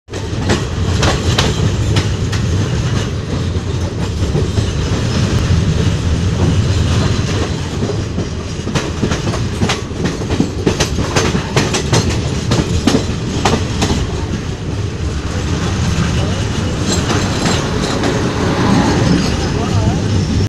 Amazing India war tankers on train rare scene
On my casual journey a rare war tankers surprised me a lot take look at indian war tankers..